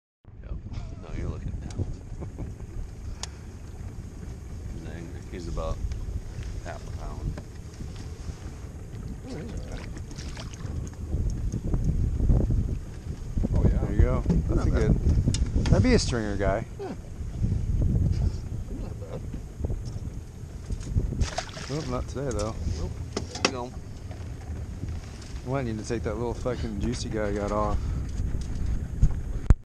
6/2/19 Glitter Lake, ON. Northern wilderness Outfitters
Dave catching a walleye